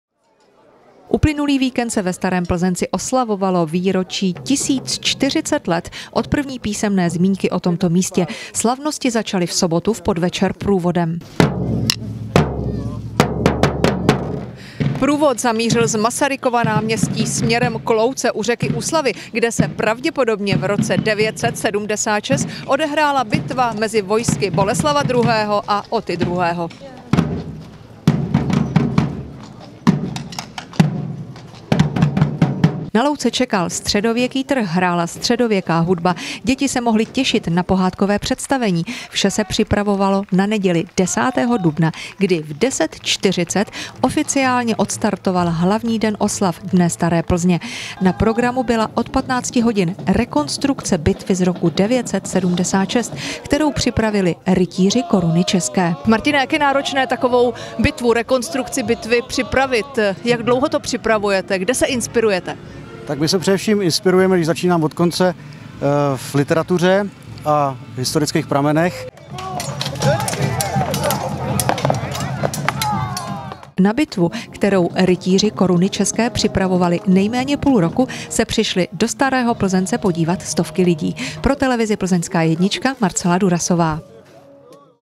Ve Starém Plzenci proběhla bitva
Uplynulý víkend se ve Starém Plzenci oslavovalo výročí 1040 let od první písemné zmínky o tomto místě. Slavnosti začaly v sobotu v podvečer průvodem.